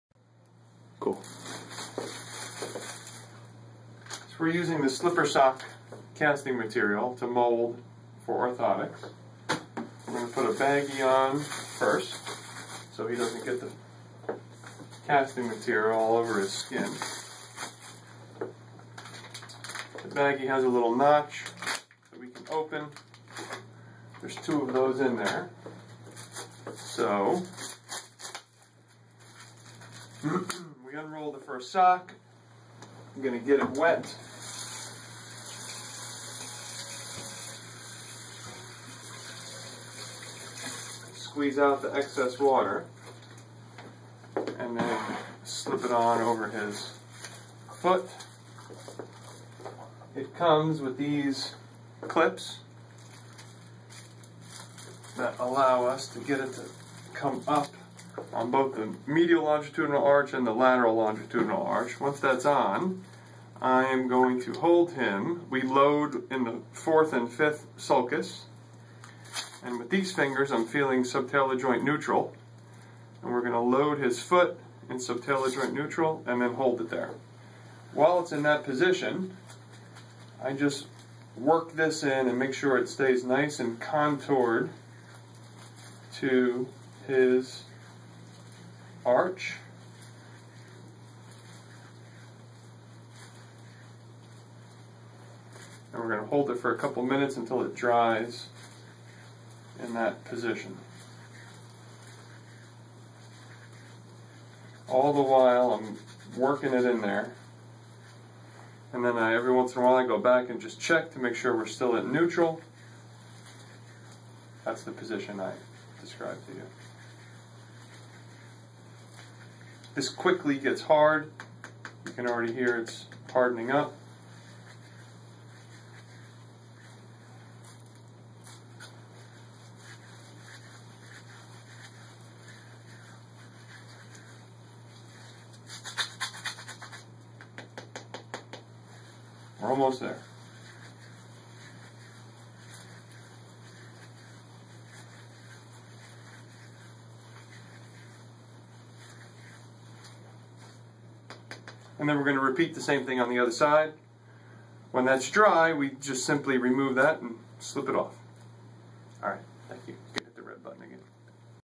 Slippersock Lehrman
Dr Jeffrey Lehrman uses Slippersock to create a mold for a functional rigid orthotic.  Dr Lehrman is a Board Certified Surgeon as a Diplomate of the American Board of Podiatric Surgery and is a Fellow of the American College of Foot and Ankle Surgeons.

podiatry
orthotic
orthotics